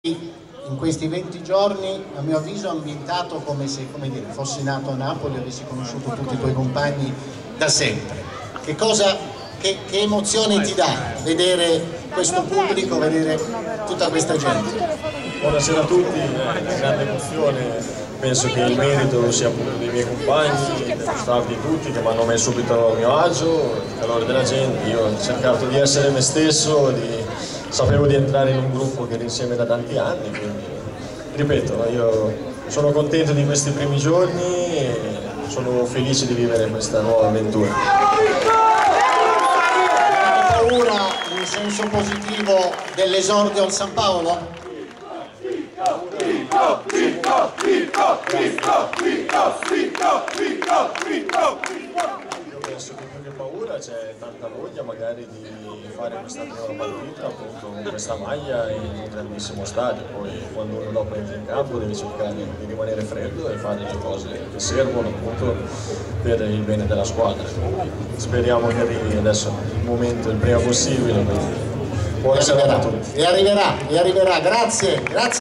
SPECIALE DIMARO - [Video Ilnapolionline] Valdifiori: "San Paolo? Non vedo l'ora"
Il nuovo regista azzurro non sta nella pelle in vista della nuova stagione di Serie A e risponde così alle domande di Lombardo.